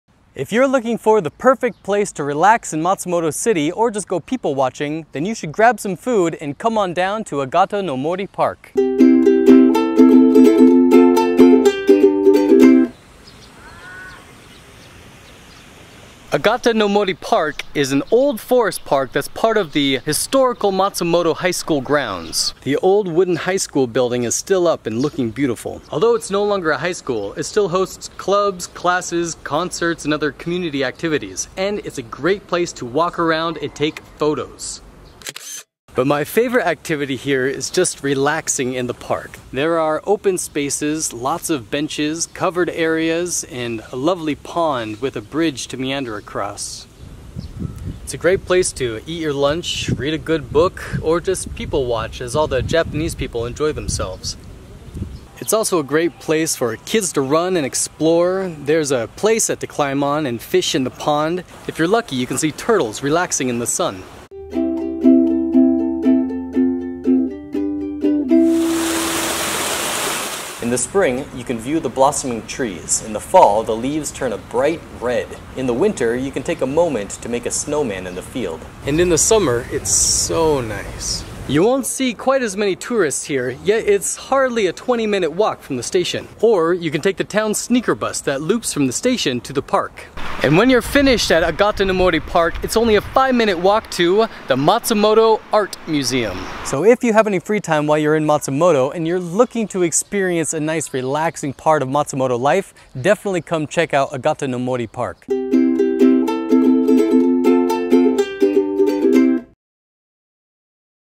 Agatanomori Park - Quick Review
Agatanomori Park in Matsumoto, Japan, is very much a locals’ park. This forest park is a great place for families, couples, or people who want to relax in a peaceful and beautiful park. You’ll see animals, ponds, waterfalls, old buildings, and tall trees. You can also learn a bit about Matsumoto history by touring the historical Matsumoto High School building.

Best part: It’s a relaxing place that’s great for tourists yet is designed for those living in the area with lots of nature, amenities, and places to play or relax.

How to get there: Walk less than 20 minutes directly east from Matsumoto Station or take the Sneaker Bus to the park.

When to go: This is a beautiful park with lots to offer all year round. I recommend going on a nice warm day so you can see some animals, enjoy a snack, and play.

Kagoshima Ramen Sakurajima (鹿児島ラーメン桜島)
Agata No Mori Tea Room (inside the Former High School Memorial Hall)